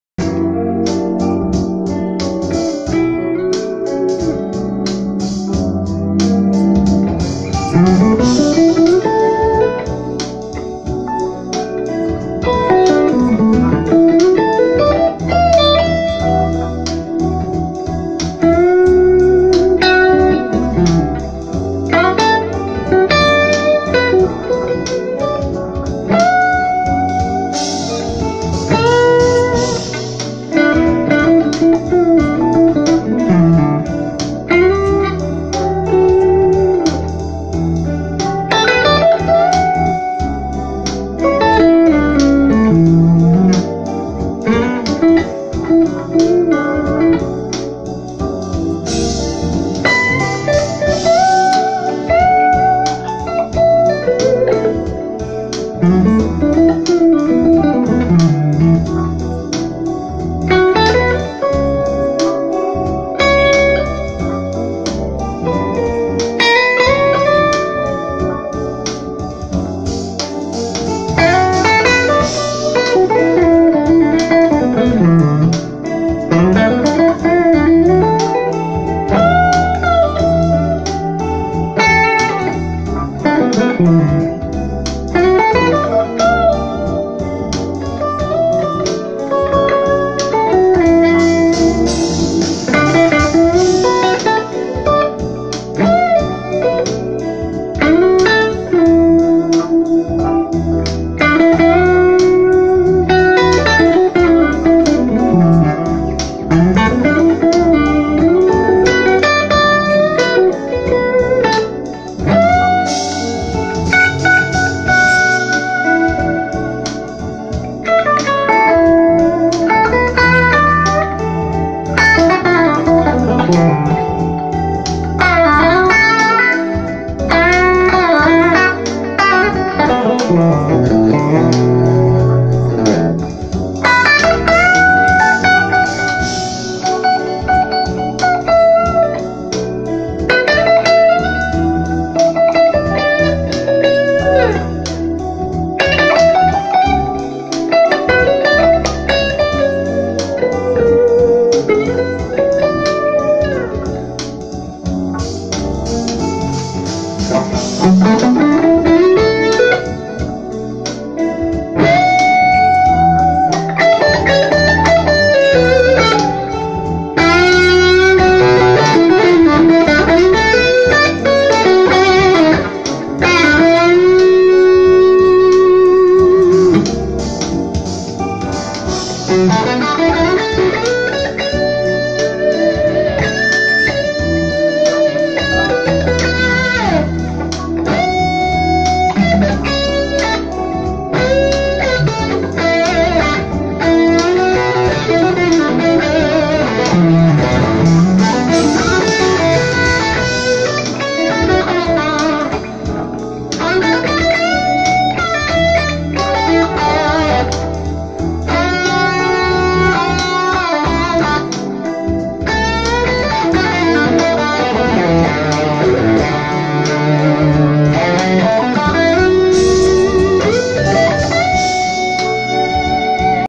My precious Kramer
Kramer Pacer Imperial II
Roland Cuibe 80 xl - Jazz Chorus clean
Boss CS3 compresor
Boss BD2 - Waza Craft - blues driver
Boss DD3 - digital delay